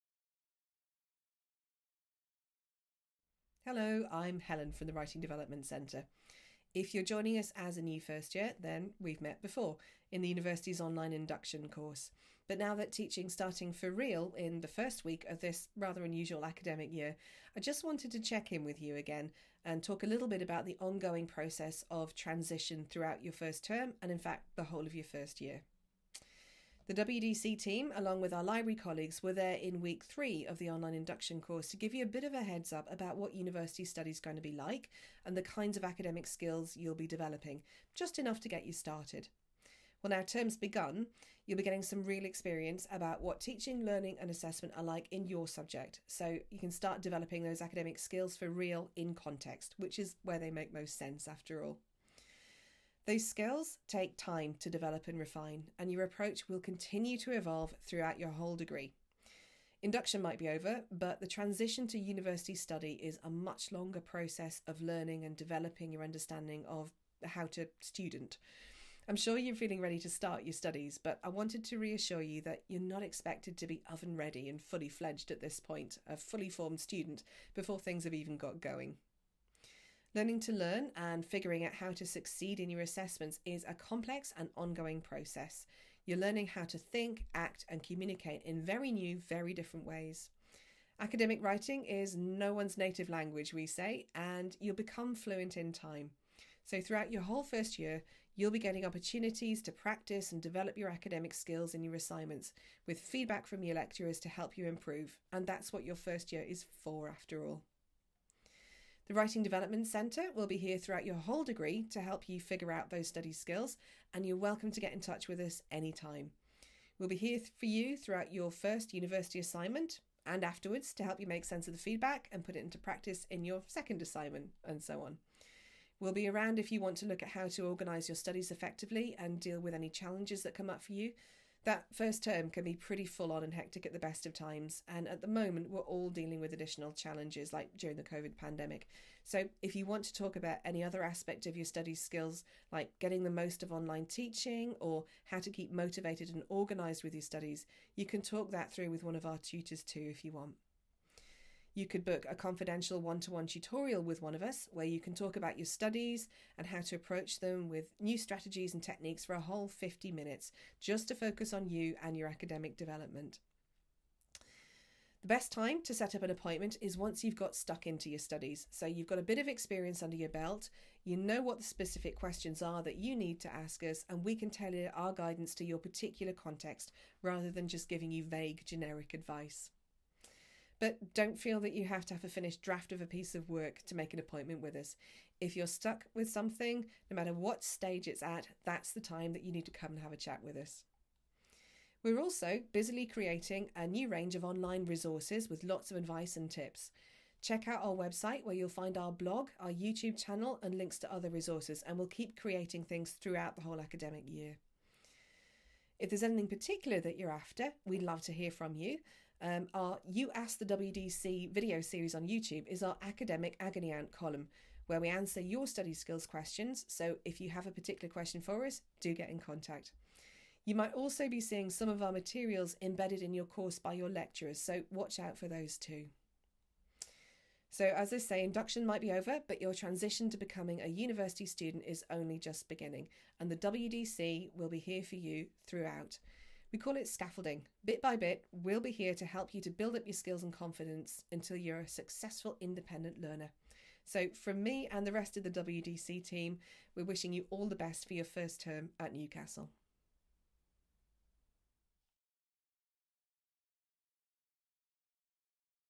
First year, first term, first week - a message from the WDC!
If you're a new first year, you'll have completed the university's online induction and heading into the first week of teaching for real. We at the WDC wanted to check in with you again to let you know that developing your academic skills is an ongoing process, and we'll be here for you throughout your first term, first year, and in fact for the rest of your studies!